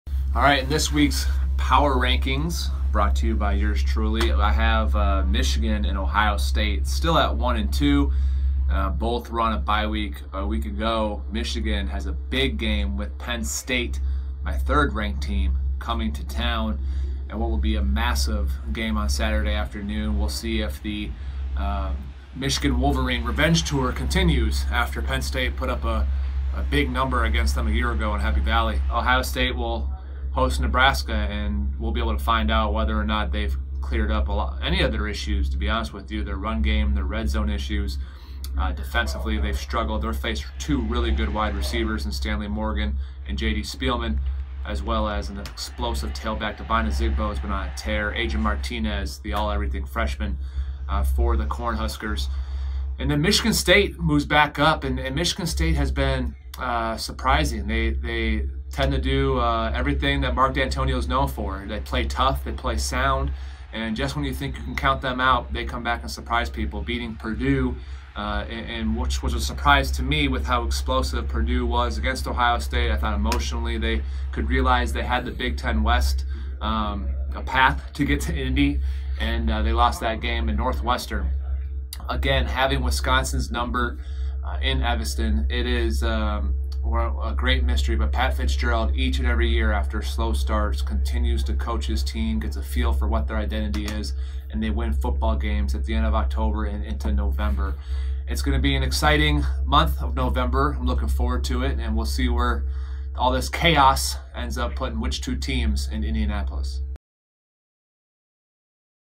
B1G Power Rankings - Post-Week 9 | James Laurinaitis |Big Ten Football
BTN football analyst James Laurinaitis gives his weekly power rankings.